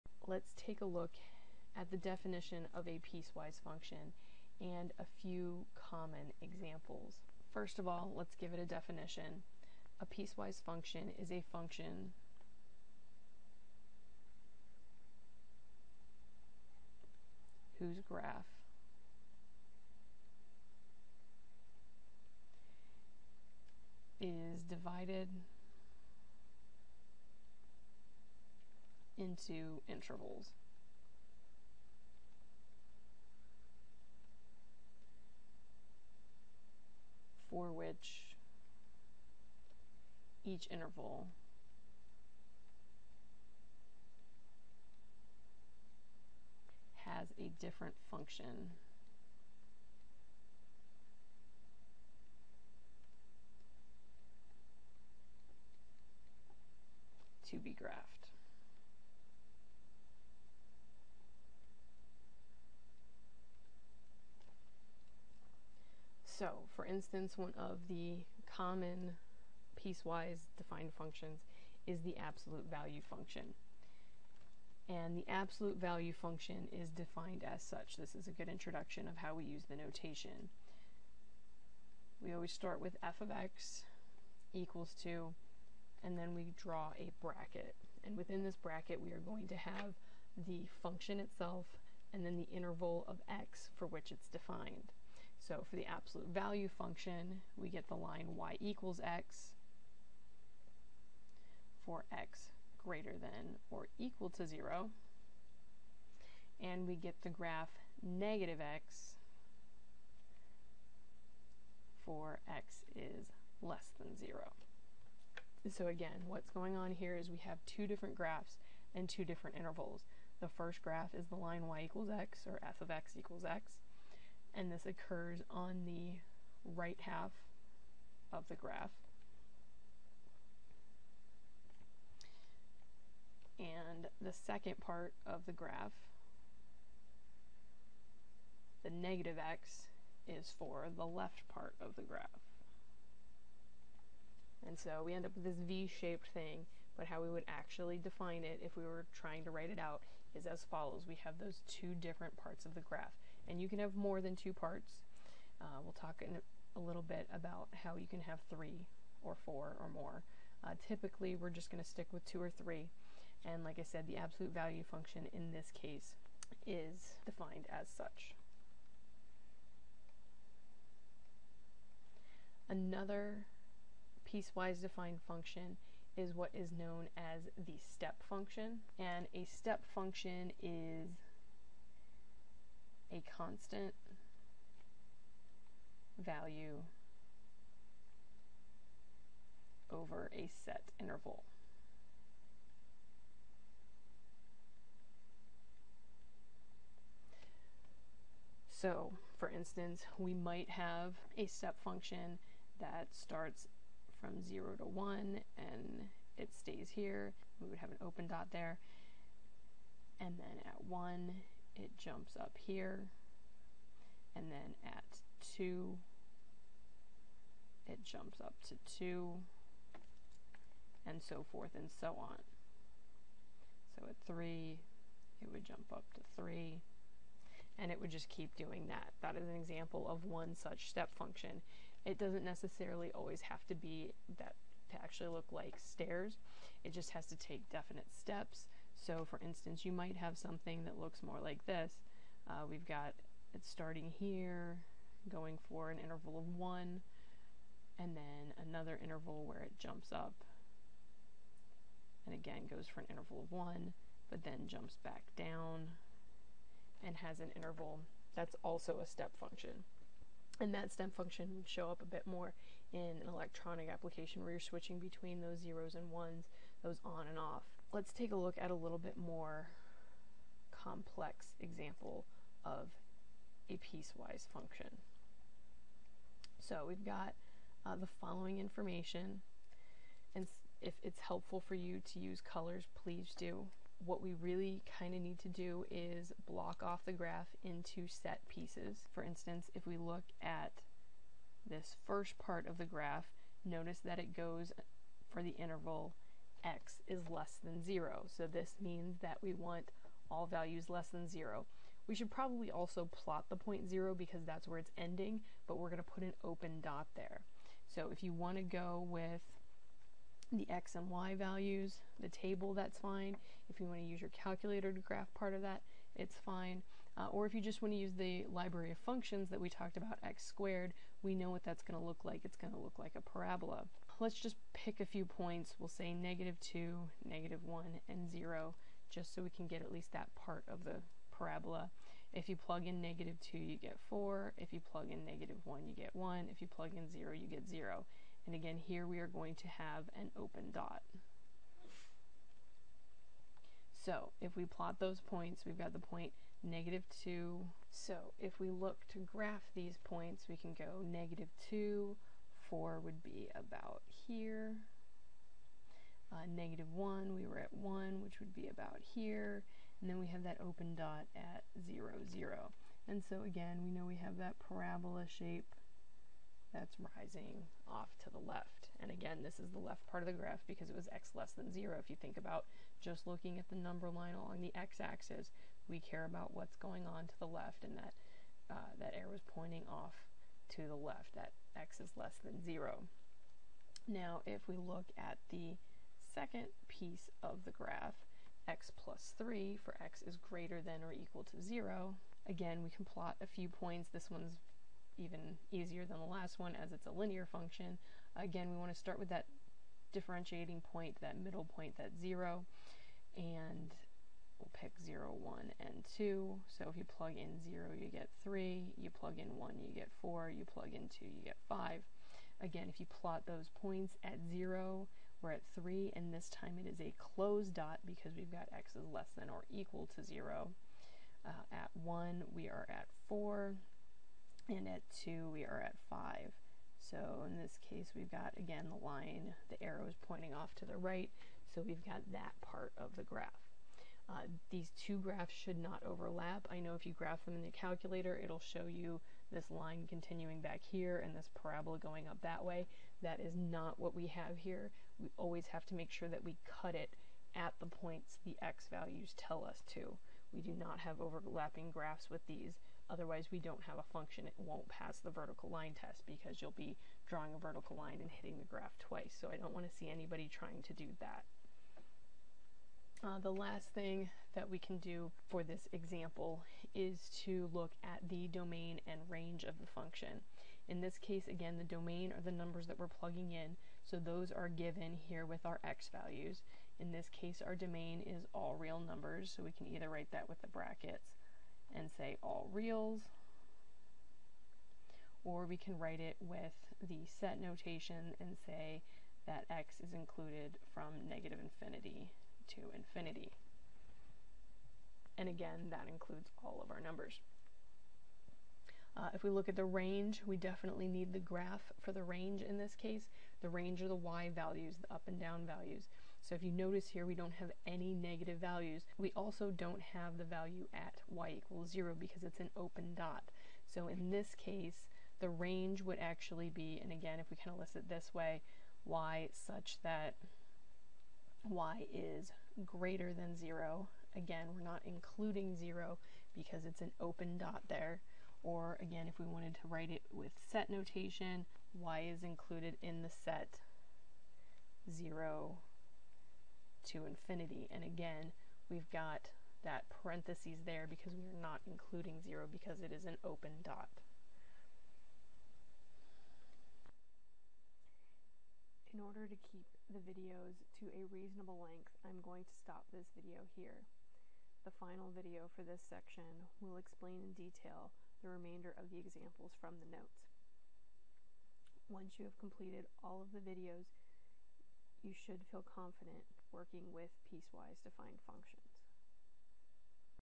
Tech Math IV: Section 1.4b
Graphing piecewise defined functions part 1.